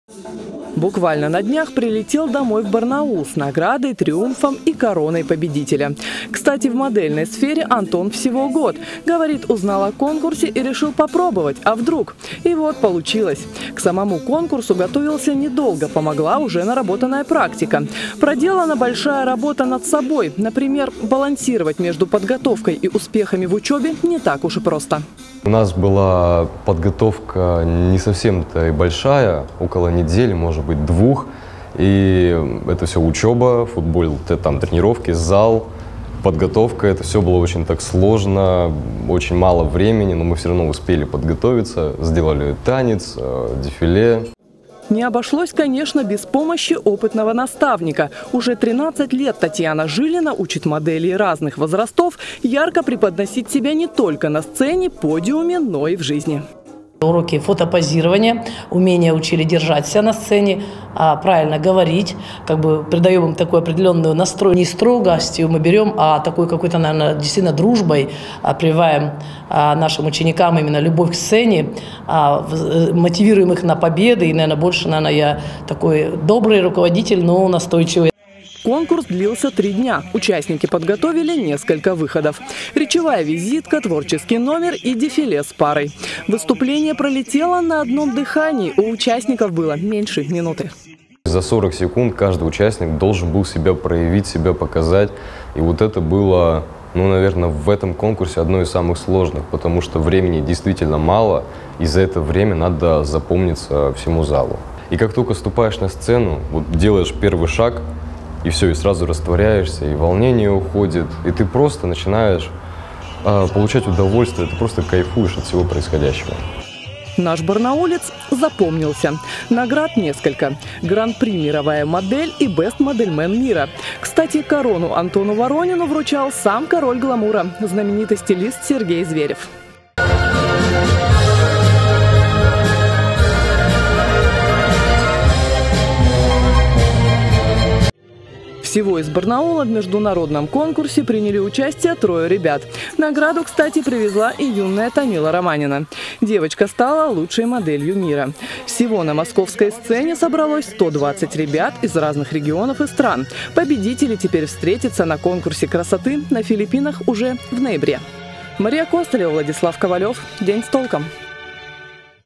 Барнаулец получил гран-при международного конкурса
Барнаулец Антон Воронин победил в Международном конкурсе красоты «Мировая модель — 2024» в Москве. Теперь он будет представлять нашу страну на конкурсе красоты на Филиппинах.